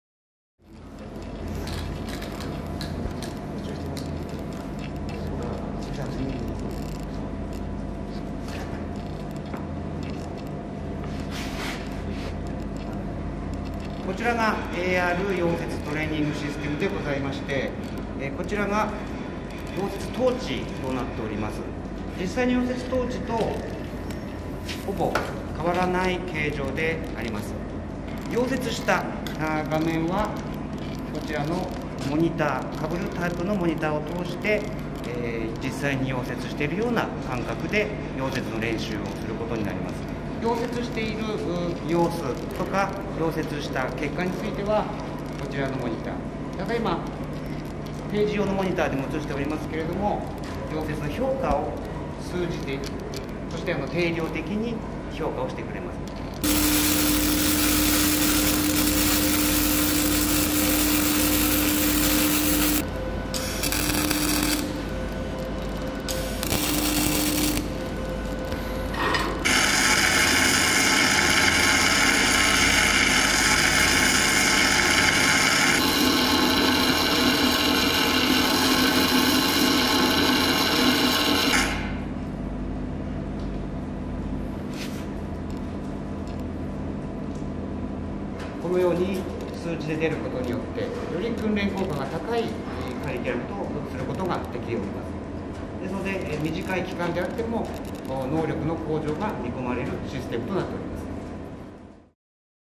溶接技術、ＡＲで安全に！ 職業訓練用に新システム
再就職希望者への職業訓練を行うポリテクセンター秋田（秋田県潟上市、中西裕之所長）は、建設現場などで用いられる溶接作業を拡張現実（ＡＲ）技術を使って練習できる新システムを県内で初めて導入した。本来は高温を生じる溶接作業を、安全に模擬体験できる。練習時の操作精度を解析・採点する機能も備えており、技能向上に役立つという。